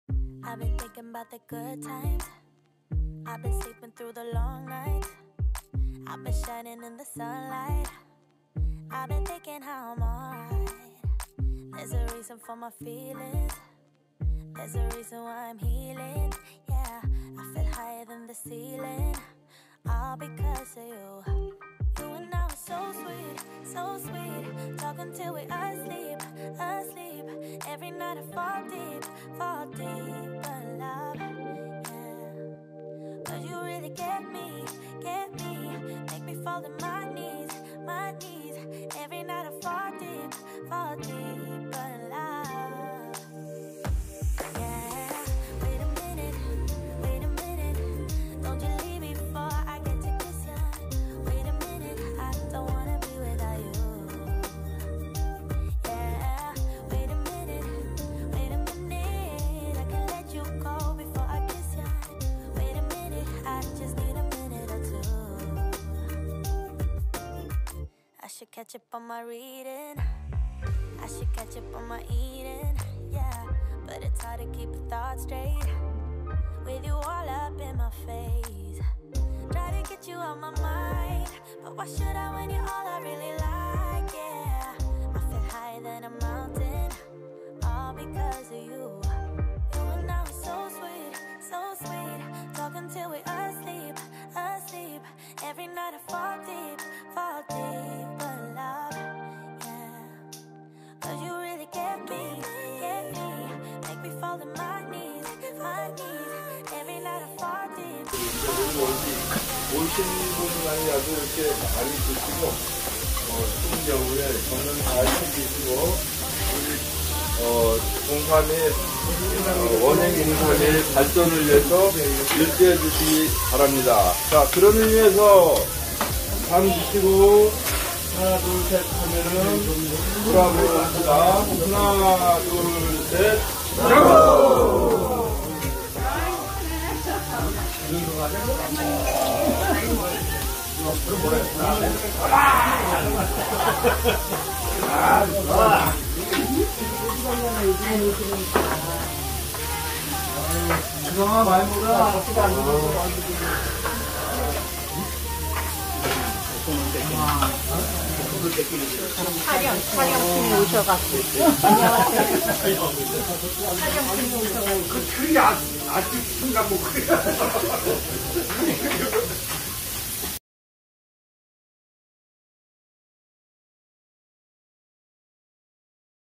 송년회 22305편
언제 어디서 만나도 하하호호~
늘 즐겁고 행복한 시간속에서
이야기 보따리를 푸는 죽마고우들과
함께 송년모임을 하였습니다.

원주의 명동이자 대표적 먹거리촌인
단계택지에서 올 한해를 정리하고
새로운 마음과 각오로 계묘년 새해를
맞이할 다짐을 해봅니다.